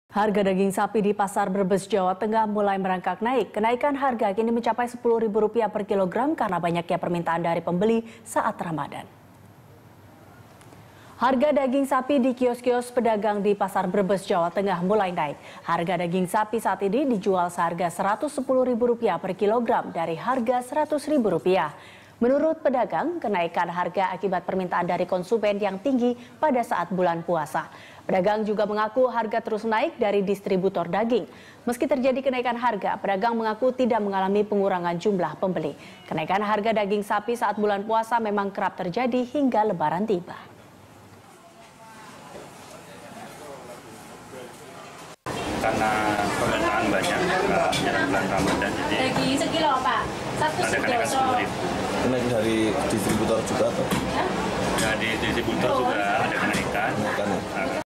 Naik, Harga Daging Naik Rp 10 Ribu Per Kilogram
Harga daging sapi di Pasar Brebes, Jawa Tengah, merangkak naik. Kenaikan harga mencapai Rp 10 ribu per kilogram.

Facebook.com/BeritaSatuTV
Youtube.com/BeritaSatu